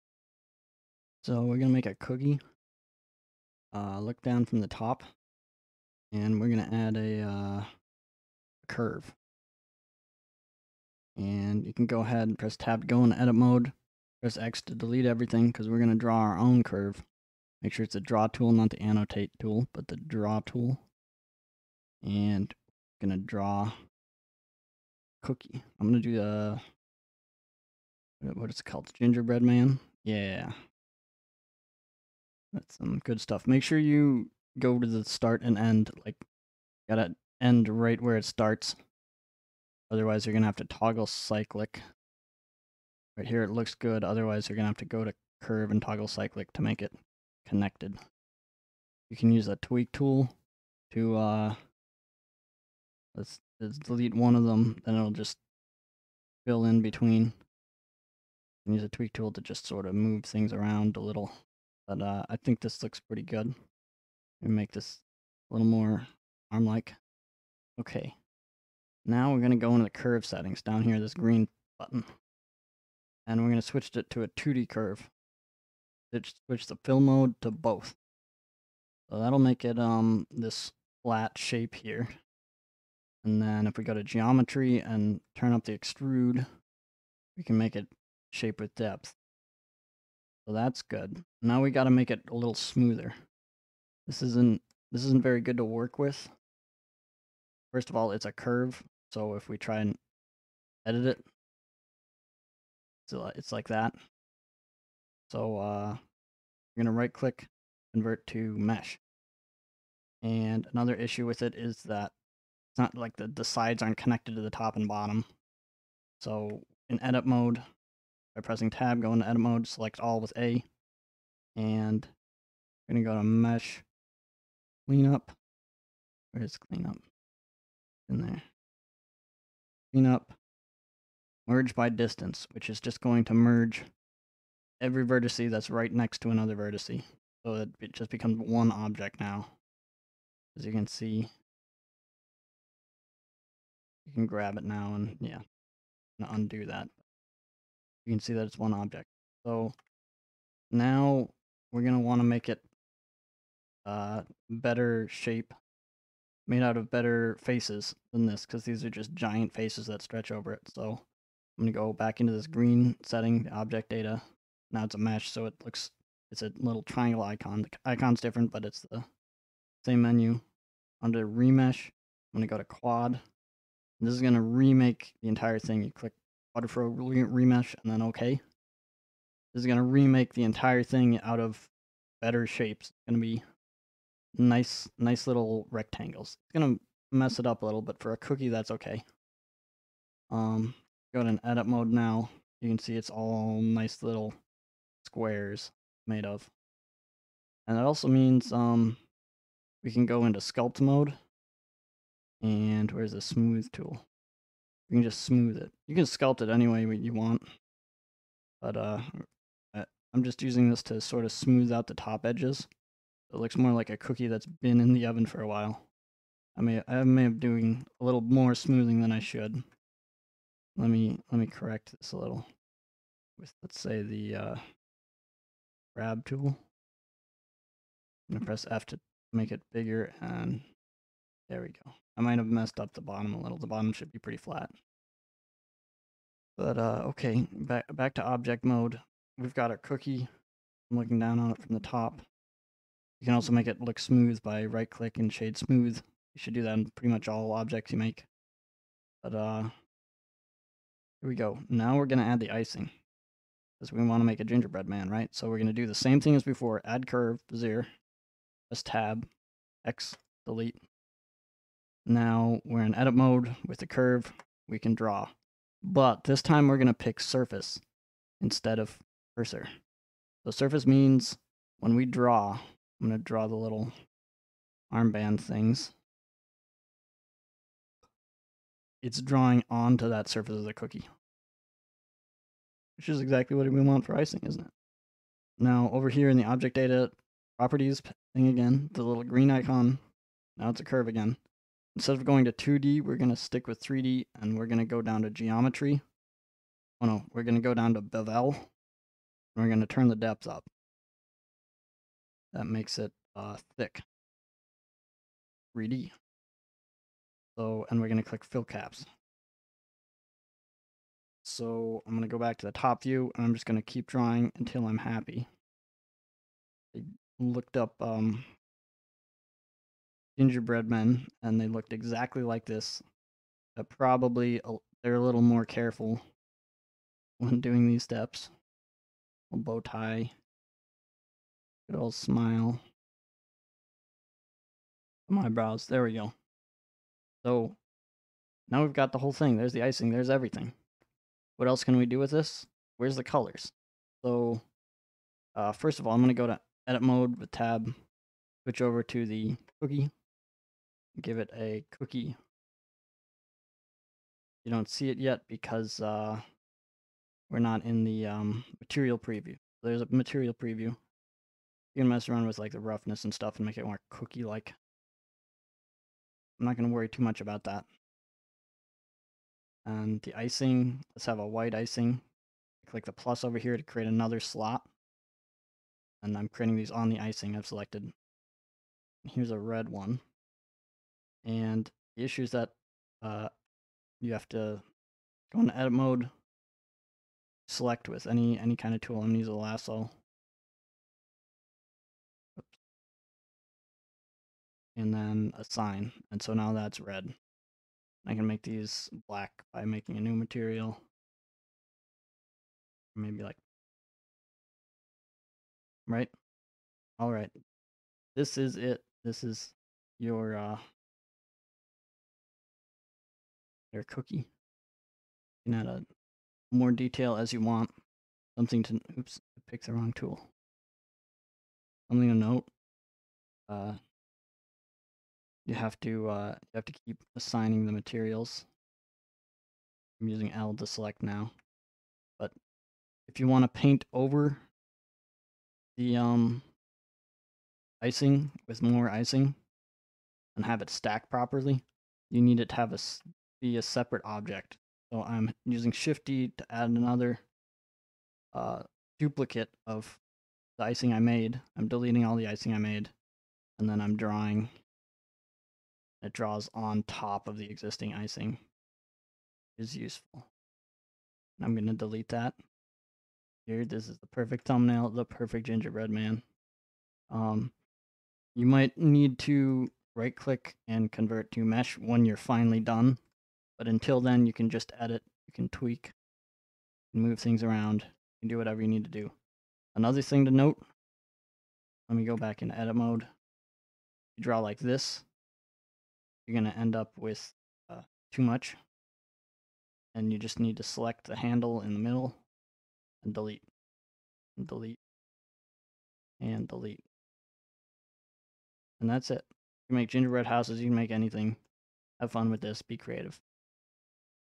Draw Icing on Cookies in 3D [Blender]
Blender's curve drawing features allows you to easily draw lines onto 3d objects. One way to use this feature is to decorate own gingerbread men or houses.